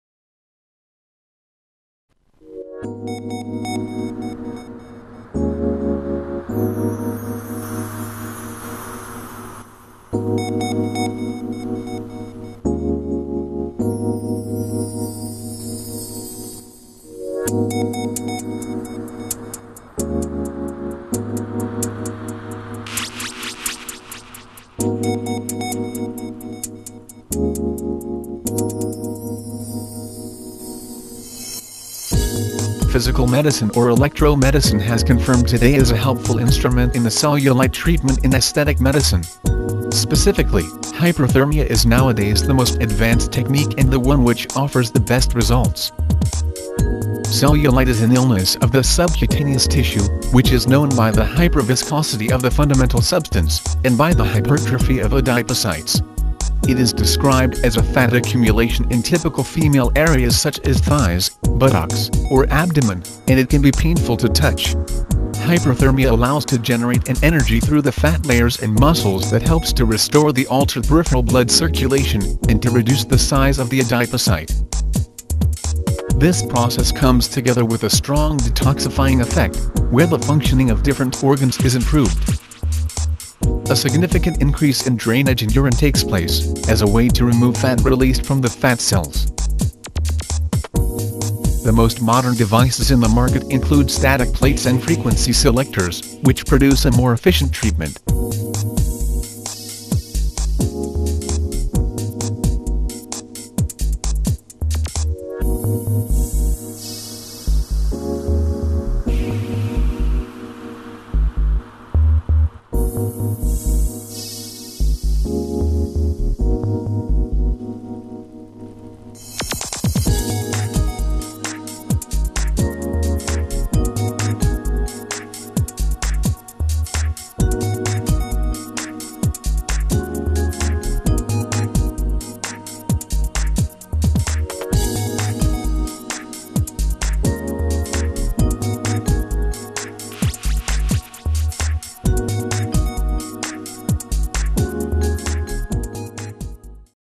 Hyperthermia in cellulite
Capenergy tecartherapy equipment helps restore impaired peripheral circulation and reduce the size of the adipocyte. See how this treatment improves both cellulite and the inicial disorders which produce it.